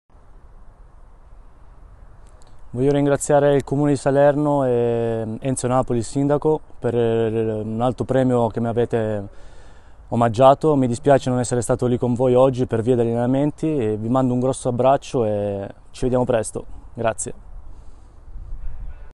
Parigini ringrazia i salernitani
Dall'ufficio stampa dell'Ascoli arriva questo video di ringraziamenti da parte di Vittorio Parigini. Nel match di Ascoli, in cui la Salernitana conduceva per la gara 2-0, si accasciava al suolo Patryck Dziczek privo di sensi. Il suo intervento, unito a quello di Francesco Di Tacchio, salava la vita al giovane polacco.